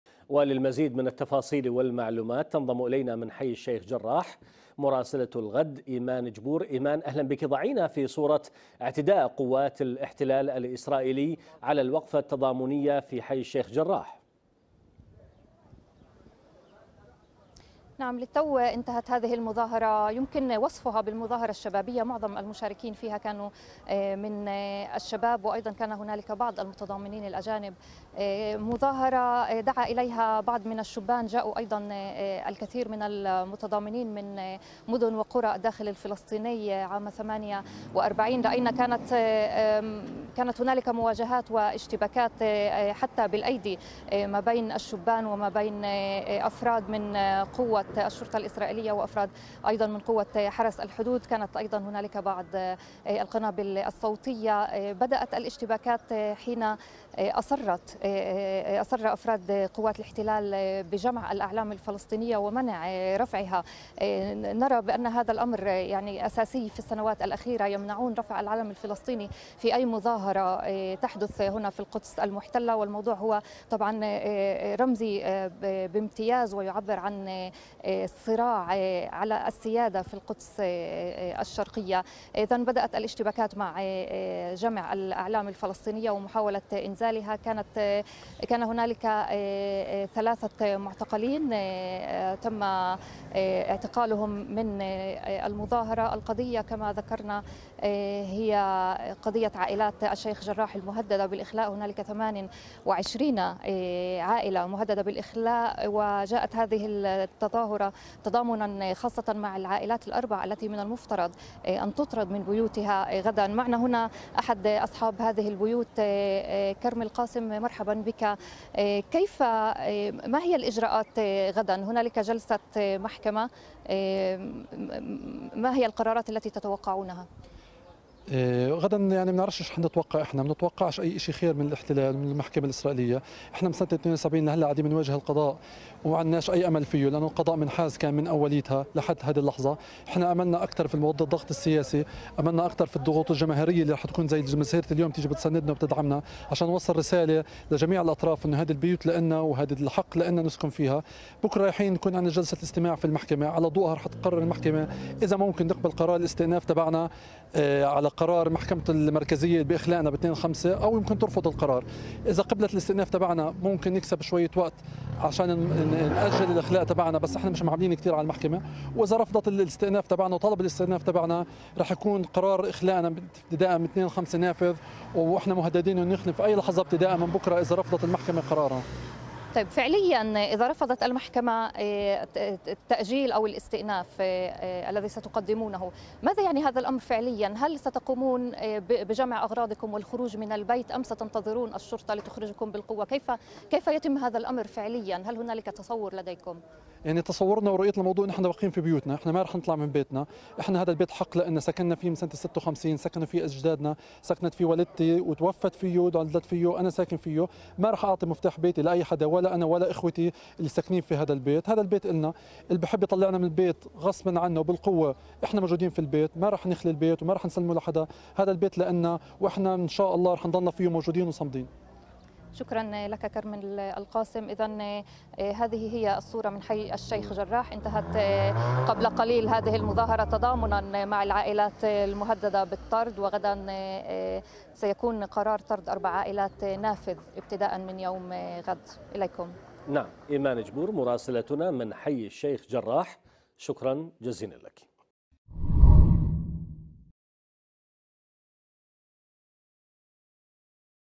عائلات فلسطينية مهددة بالطرد من منازلها بالقدس.. صاحب أحد البيوت يروي للغد تفاصيل ما يجري
أطلقت فعاليات شبابية ومؤسسات مقدسية حملة تضامن مع أهالي حي الشيخ جراح في القدس المحتلة وذلك بعد ساعات من قرار سلطات الاحتلال بطرد أربع عائلات من منازلها بقرار من محكمة الاحتلال، وشارك المئات من المقدسيين في وقفة احتجاجية امام البيوت المهددة بالإخلاء رفعوا خلالها الشعارات المنددة بقرار سلطات الاحتلال تهجير عشرات العائلات  من الحي.
المزيد من التفاصيل مع مراسلة الغد من القدس المحتلة إيمان جبور عبر النشرة الإخبارية على شاشة قناة الغد.